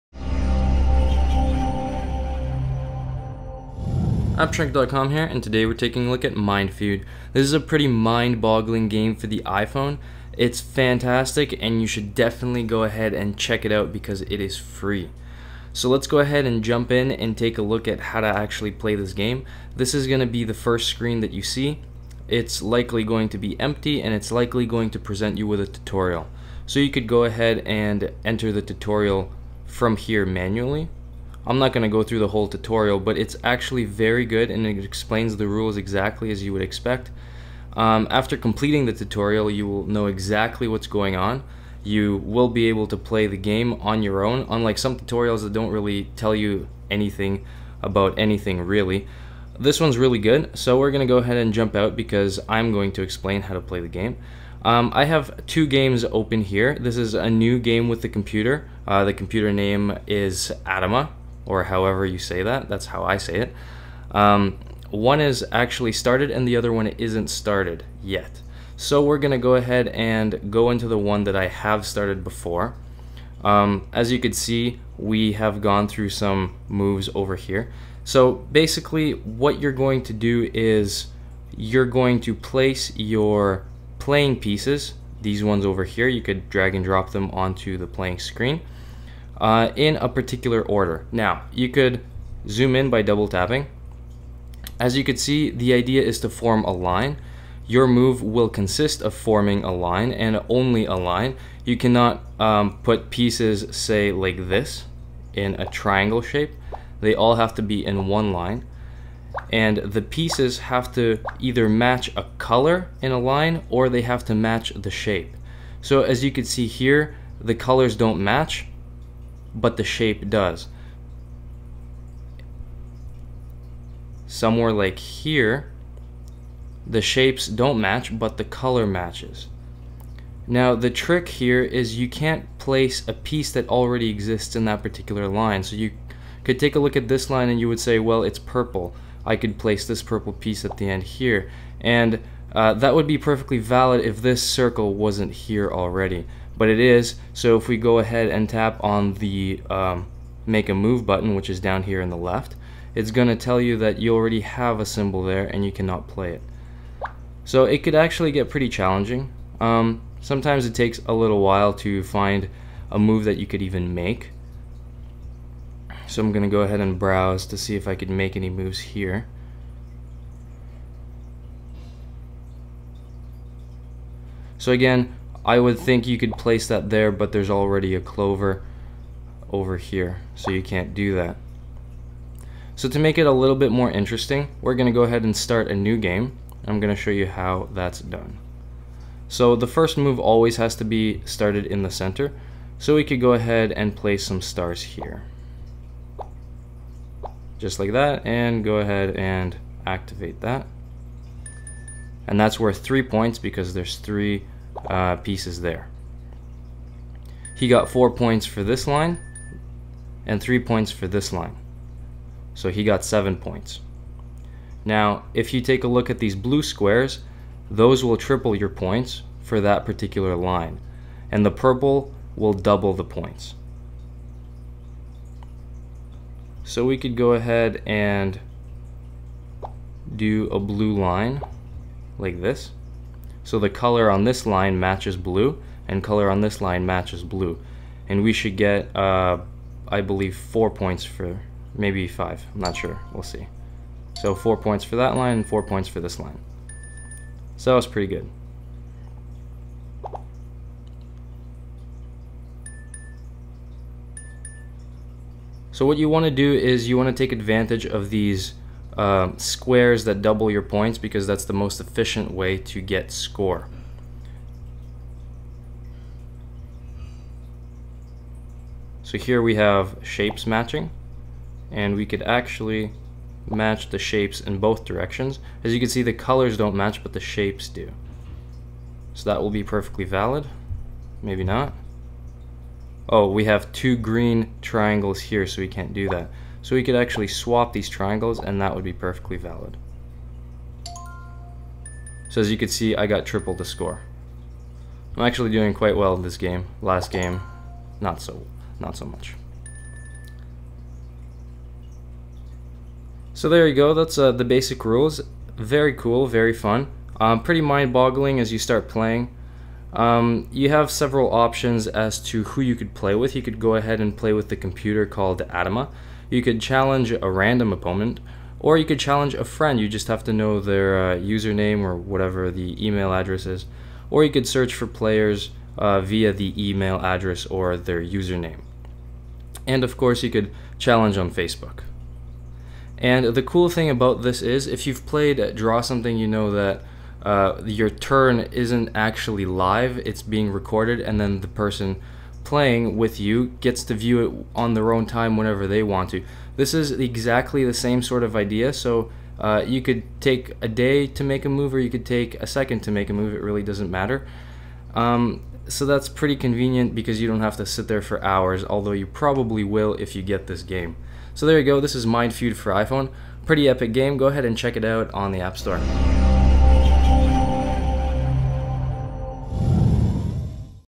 MindFeud for iOS - Game Review
If you are looking for a game that will challenge your logical thinking, provide large amounts of fun and pleasant frustration, then MindFeud is a must have game for your iPhone or iPad. Available for free as a Universal app.